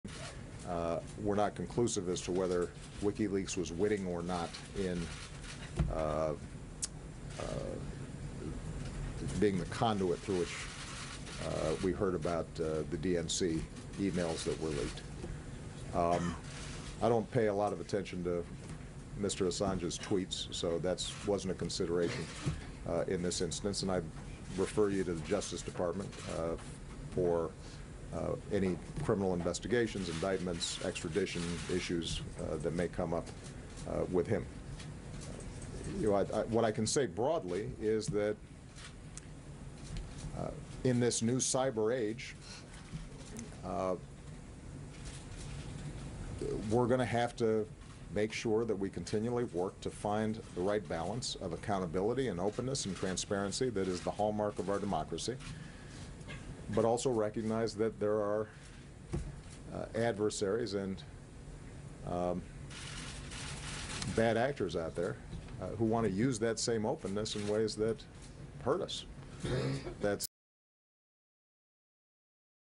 2017 01 18 Obama WikiLeaks
Pres. Obama referrers to DNC intrusion as leak vice hack.